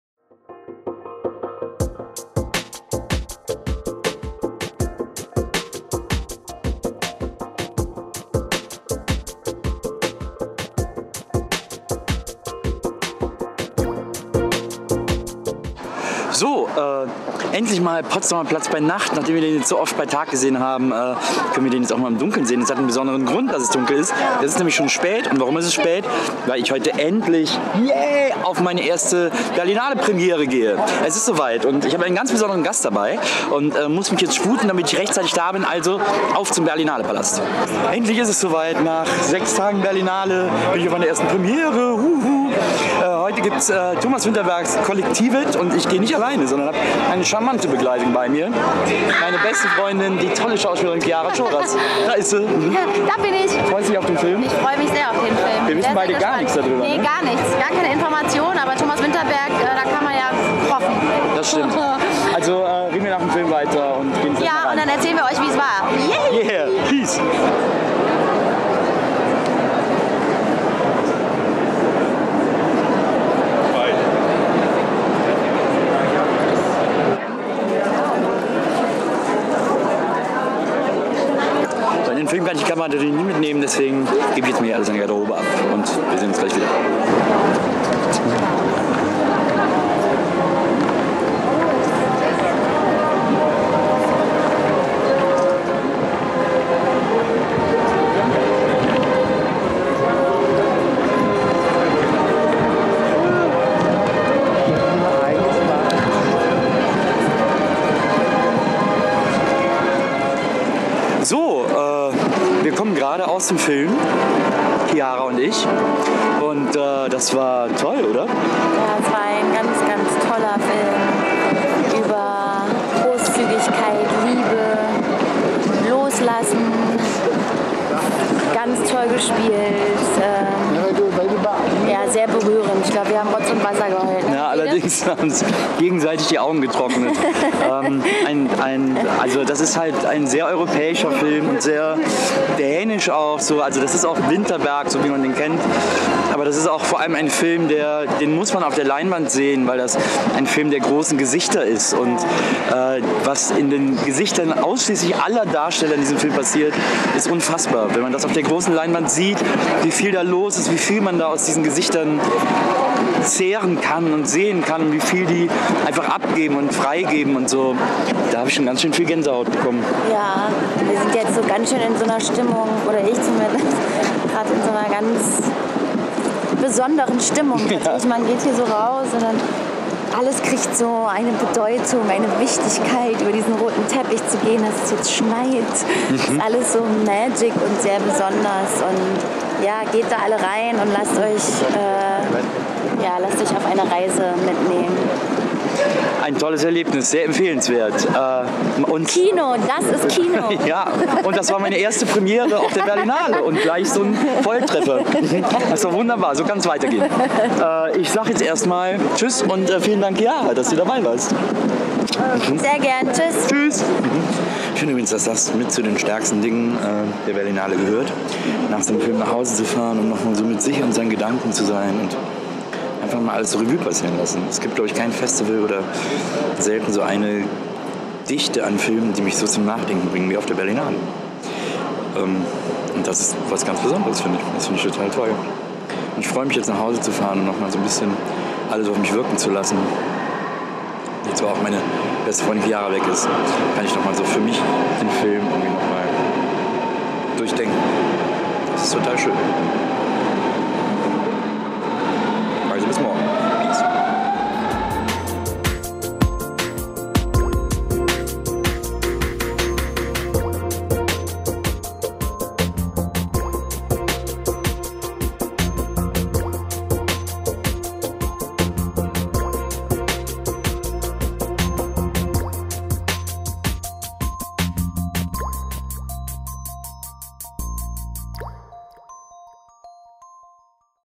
Berlinale Uncut mit Nilz Bokelberg - Tag #7
Langsam wird es mal Zeit für eine Berlinale-Premiere! Und deshalb läuft Nilz heute, gemeinsam mit seiner besten Freundin, über den Roten Teppich des Berlinale Palast und sieht sich Vinterbergs "Kollektivet" an.

Präsentiert von kino-zeit.de in Zusammenarbeit mit MasterCard, offizieller Sponsor der 66. Internationalen Filmfestspiele Berlin.